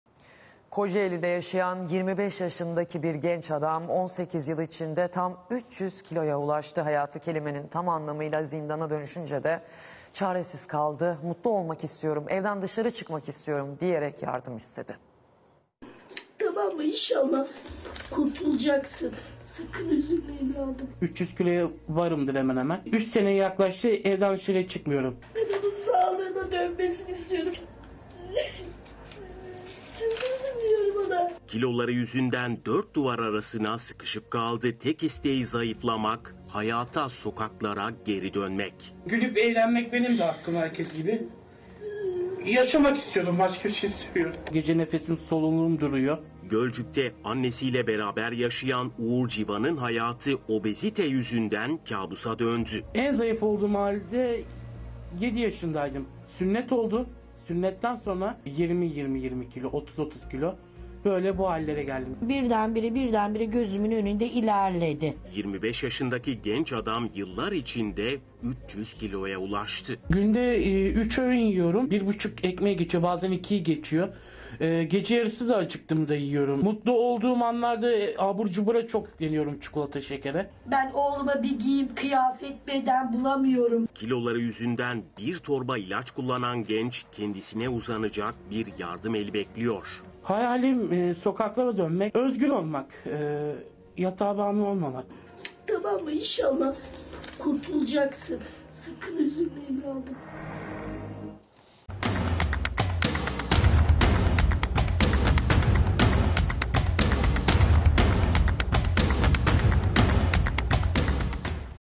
25 yaşında ve 300 kilo!
KOCAELİ'DE YAŞAYAN 25 YAŞINDAKİ GENÇ ADAM 18 YIL İÇİNDE 300 KİLOYA ULAŞTI.  HAYATI KELİMENİN TAM ANLAMIYLA ZİNDANA DÖNÜŞÜNCE DE ARTIK ÇARESİZ KALDI... "MUTLU OLMAK İSTİYORUM, EVDEN DIŞARI ÇIKMAK İSTİYORUM" DİYEREK YARDIM İSTEDİ.